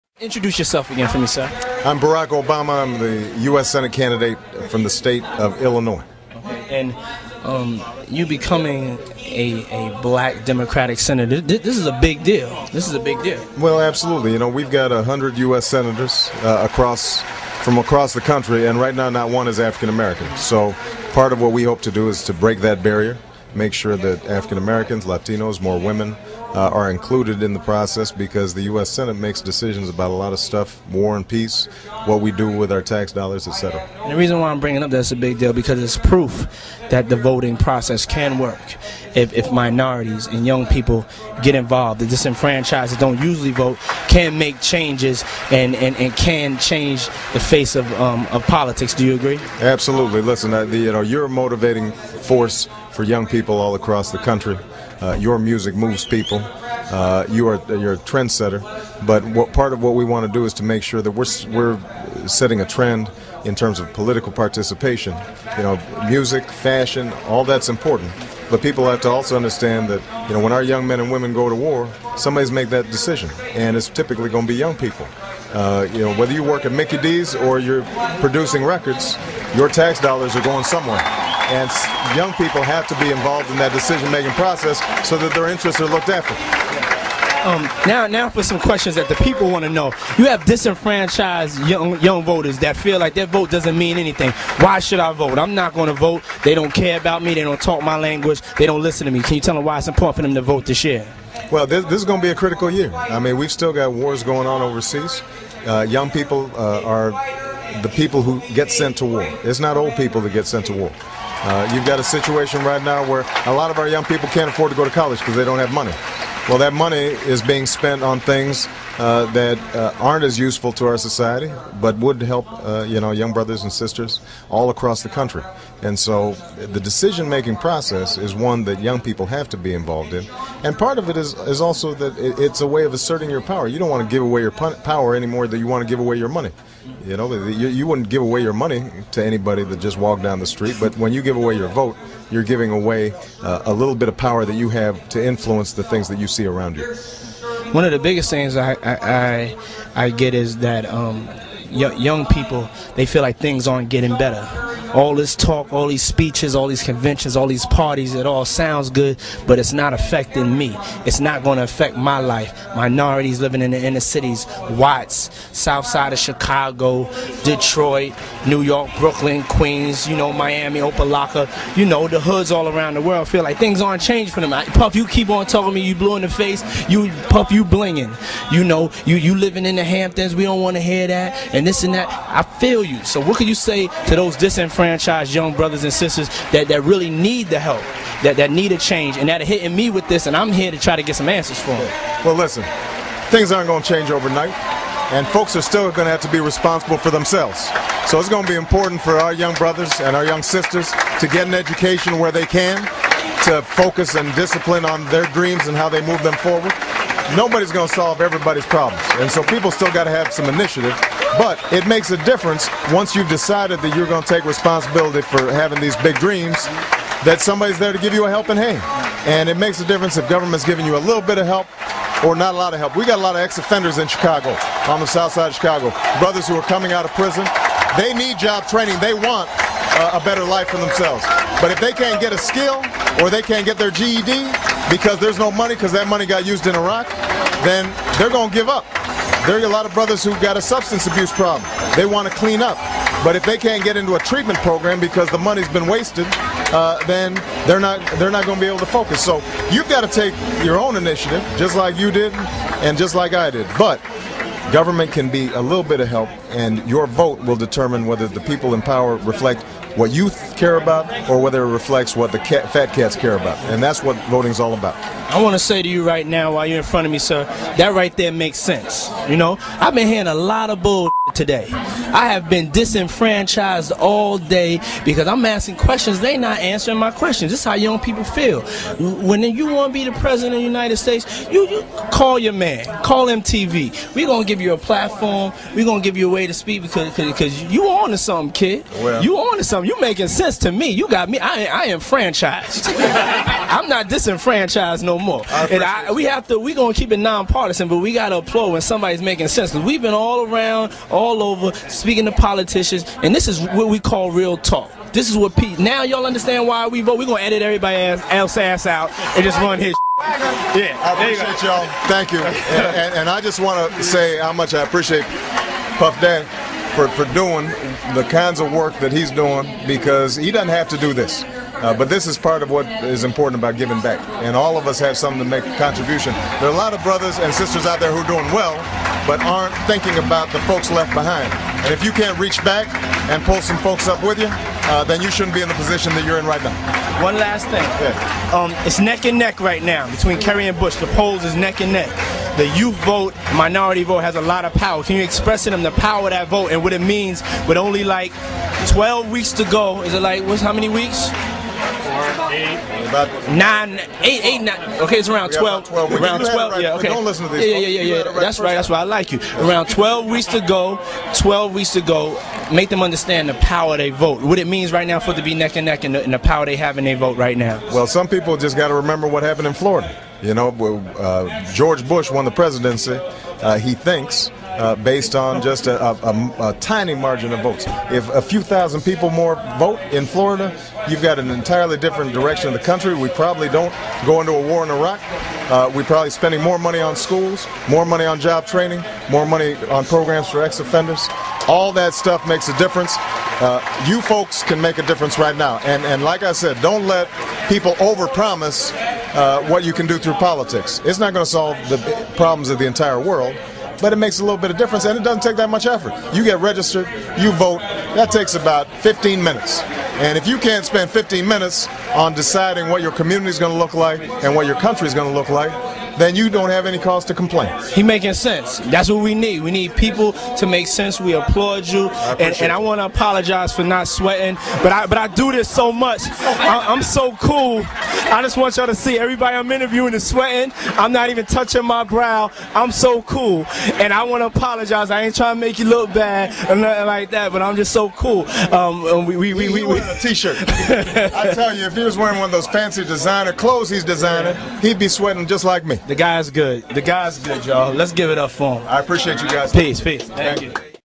Barack Obama and Diddy interview (2004)
Sean Combs interviews Barack Obama, US Senate candidate, in the summer of 2004